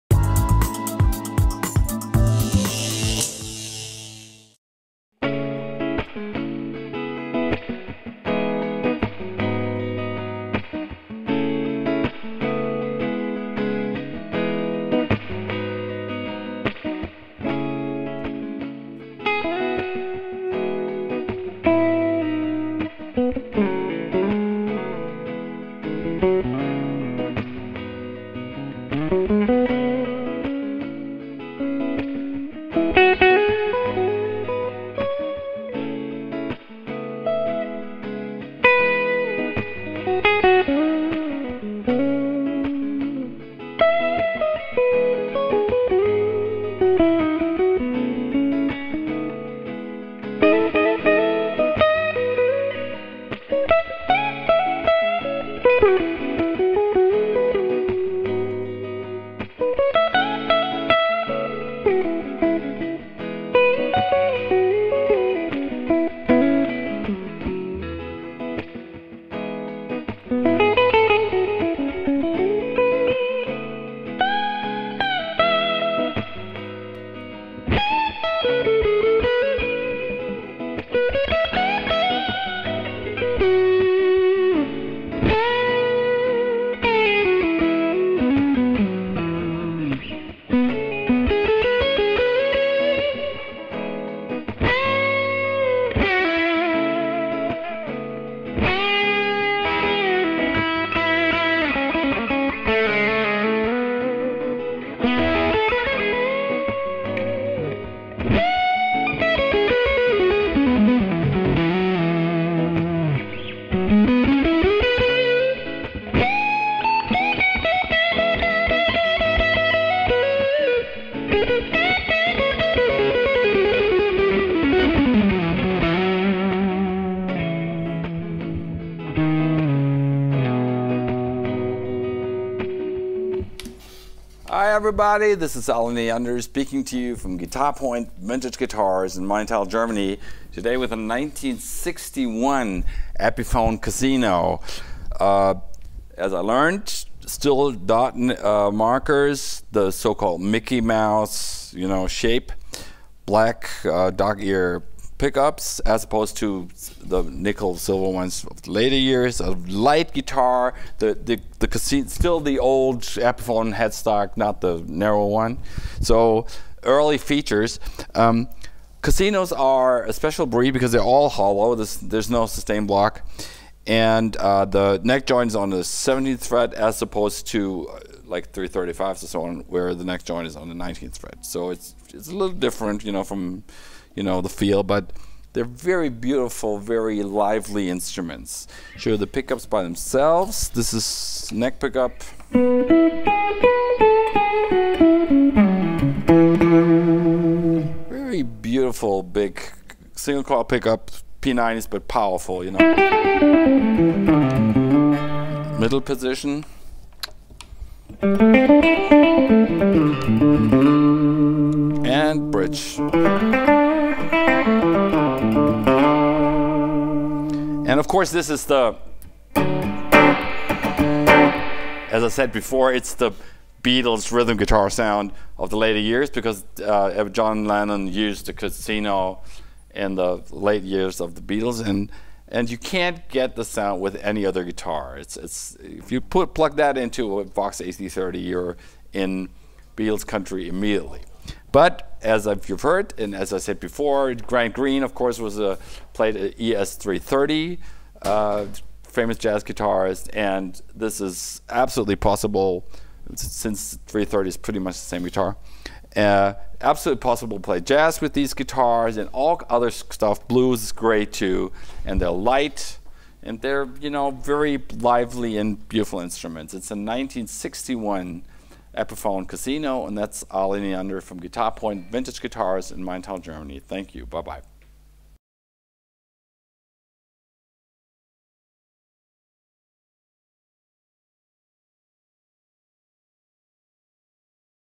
1961 Epiphone Casino - Sunburst / GuitarPoint Maintal / Vintage Guitars
1961 Epiphone Casino - Sunburst inkl. orig. case
Super clean and all original 1961 Epiphone Casino in exellent/- near mint condition! Almost like new, only the fact that someone stitched his Passport-number(?) in the back of the headstock keep this Instrument from beeing a "Minty", ...still best condition we´ve seen a Casino ever! 1961 was the firstyear for the Casino, with a smaller headstock, "Dot-Inlays", brown tortoise pickguard and black plastic PU-covers (Dog-Ears!), those ´61-´63 models are comonly known as the Keith Richards-models, ´64-´65 versions known as the Beatles-models. The guitar has barely been played, no visible playwear, no weatherchecking on the finish and comes with some case-candy. It comes with the wider pre-/65 neck very comfortable to play and the two P-90 sounds superb! The comes with its original grey Epiphone hardshell case w/blue interior and some original case-candy.

Techn. details:

    16" Thinline Hollowbody (335-Style) - Doublecutaway,
    Bound 1-piece mahagony neck w/small headstock,
    6x Kluson "Single-line" tuners w/orig. plastic buttons,
    Rosewood fretboard with 22 frets and dot-Inlays,
    2x AlNiCo V P-90 Pickups w/blk dogear covers,
    2-Volume and 2-Toneregler,
    4x orig. golden reflector knobs,
    Single-ply brown tortoise pickguard,
    Epiphone Trapeze" -Tailpiece,
    Orig. Gibson ABR-1 "no-wire" bridge,
    Nickel-hardware,
    Incl. original grey Epiphone HSC w/blue lining.